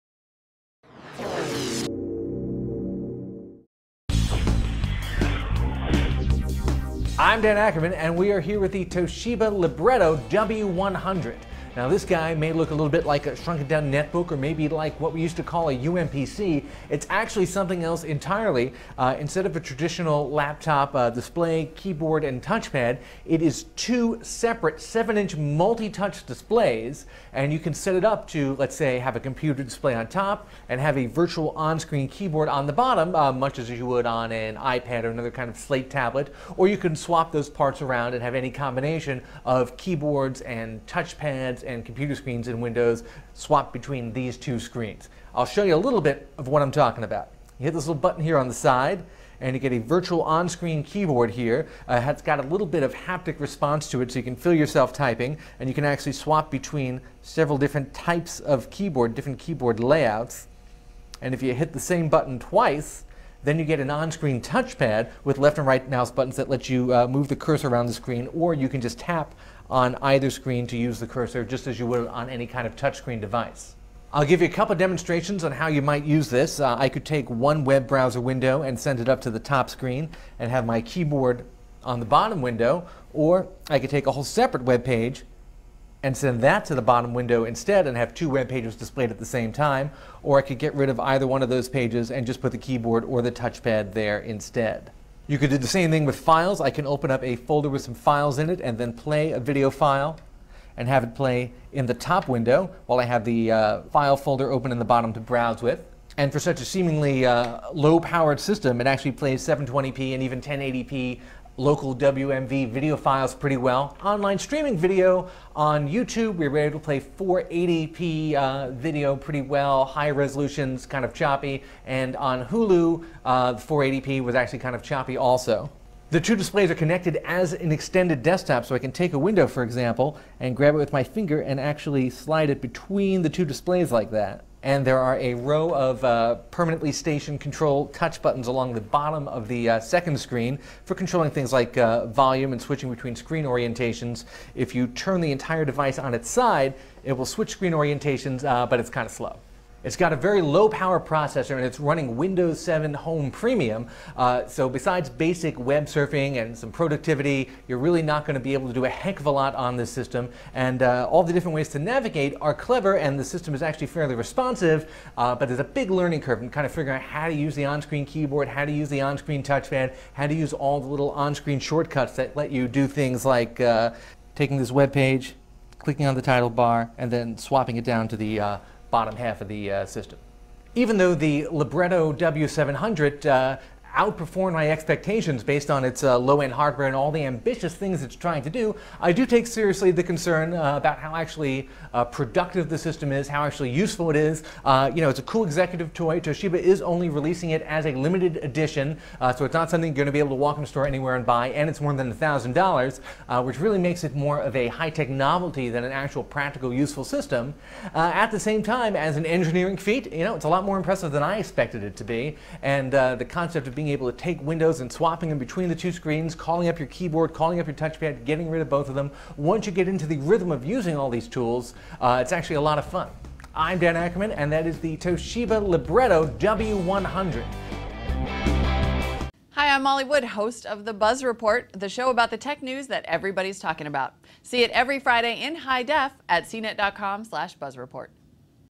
Toshiba Libretto W105 first look review
Toshiba Libretto 105-L251

Looking a little like an oversize Nintendo DS, the libretto has two 7-inch multitouch displays, with the second taking the place of the traditional keyboard one would expect to find in this kind of clamshell design. By tapping a button on the side of the chassis, a virtual keyboard (similar to what you'd find on a iPhone or iPad) pops up to fill the bottom screen. Tap the same button twice and you get a virtual onscreen touch pad instead.

Our first struggle came with figuring out how to juggle these two virtual input devices, as the bottom screen isn't large enough to display both the keyboard and the touch pad fully at once (and, in our tests, the onscreen keyboard and touch pad couldn't register inputs simultaneously). Eventually, we got into a nice rhythm of single- and double-tapping to switch from keyboard to touch pad on the fly, although it's a little counterintuitive.

Pressing the button on the right side of the bottom display switches between the standard Windows OS desktop and a series of Toshiba's proprietary Bulletin Board screens, which allow you to arrange photos and notes on a touch-friendly surface. It looks snazzy, but we can't say it's particularly useful, especially as it (like almost any proprietary app) has its own learning curve.

Actually navigating around the Windows interface was mostly lag-free, which is something even many Netbooks can't say. At the same time, a 7-inch touch screen, no matter how many navigational tricks you include, simply isn't optimal for touch, and we spent plenty of time hunting and pecking, trying to center the tiny cursor on buttons and tabs.

One of the most important navigational shortcuts is called the Easy Menu Utility. Behind this generic name is an onscreen overlay that appears when the title bar of a window is tapped. A nine-square grid overlays the display, and gives you large tap-friendly buttons for maximizing and minimizing the active window, toggling the window to the other display, or even stretching the active window to cover both displays. It's handy, but having to hide whatever you're looking at behind an overlay, even for a few seconds, suggests that some form of gesture control system might be more useful.

Unlike Apple's iPad, you can't simply use your finger to flick-scroll through Web pages or documents, and working tiny buttons and tabs was frustrating, even when using the onscreen touch pad instead of a finger. That said, the touch controls on the Libretto are easily among the most responsive we've encountered on a Windows-based tablet. Unlike some of the other WinOS tablets we've tried, you won't spend most of your time staring at the spinning Windows wait icon, and we'd compare the overall experience to a decent Intel Atom Netbook.

Video playback of local 720p video files was flawless, but online streaming video was trickier. On YouTube, 480p videos played smoothly, whereas 720p ones stuttered. On Hulu, 480p video was choppy, but the lower-res options worked well, and still looked decent on the small 7-inch 1,024x600-pixel screen. Gaming, as one might imagine, is largely out of the question, although the handful of Facebook games we tried worked fine and animated smoothly.

Toshiba Libretto 105-L251